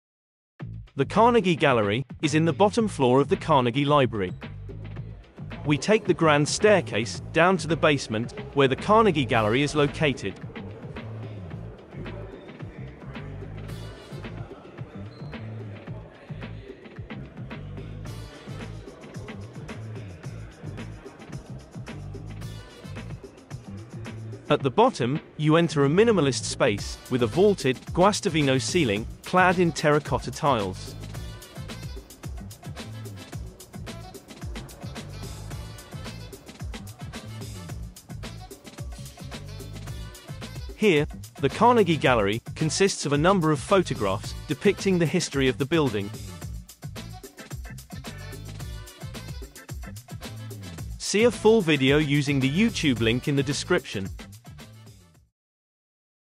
The Carnegie Gallery - at the Apple Carnegie Library in Washington DC
In case you're wondering, here is a video clip of the Apple Carnegie Library in Washington, DC. It is definitely worth a quick visit as the building is an architectural delight and has been renovated into a multi-use property tying in a modern Apple Store, the DC History Center, and a Carnegie Gallery detailing the history of the building. There is no charge for access to any of the areas, so if you are close, stop by to check it out

The Carnegie Library of Washington D.C. is located in Mount Vernon Square in Washington, DC. It is on K street between 7th and 9th Ave. It was known as the Central Public Library, but was renovated by Apple and Events DC starting in 2016 and reopened in 2019 as the Apple Carnegie Library.

Although the history of the building is somewhat tumultuous, shut down, renovated, and reopened numerous times over more than a century; it has managed to remain true to its original mission, to be a “University for the People”; A motto that is inscribed on the benches in front of the building.

Today, it hosts a flagship Apple Store which host educational events on technology, photography, filmmaking, music creation, coding, design and more; the Carnegie Gallery; and the DC History Center.

The origin of the building was actually a fortunate chance meeting between Brainard Warner who bumped into Andrew Carnegie in the White House anteroom in 1899. Warner was the president of library trustees at the time took the opportunity to ask Carnegie for a library building in Washington DC. Carnegie agreed to donate $250,000 (written on the back of an envelope) towards its construction. It was opened in 1903 and DC being a segregated city at the time, the Carnegie Library opened as DCs first desegregated public building serving everyone equally regardless of race.

As one can see, the Carnegie Library is a masterpiece of design and architectural ornateness. The design was created by the New York firm, Ackerman & Ross; who won out of 24 competing designs submitted in a contest held by The Washington Public Library Commission.

Ross’s design was inspired by his training at the École des Beaux-Arts in Paris, and while the library looks like many of the buildings in Washington, DC today (with its symmetry, sculptural decorations, flat roof, archways, pediments, and columns), it was actually one of the first buildings in DC to use this design. The ornate facade and sculptures most likely required special artisans to construct the building.

As soon as it was opened, the Carnegie Library was actually too small for the 382,352 volumes it needed to manage. It was also an outdated layout, with many “closed stacks” where librarians would have to retrieve books for customers despite an area of “open shelves” for public access. It managed to operate as the central public library for Washington, D.C. for almost 70 years before it was replaced by the nearby Martin Luther King Jr. building.

Today, the building currently consists of three main areas. An Apple Store, the Carnegie Gallery, and the DC History Center.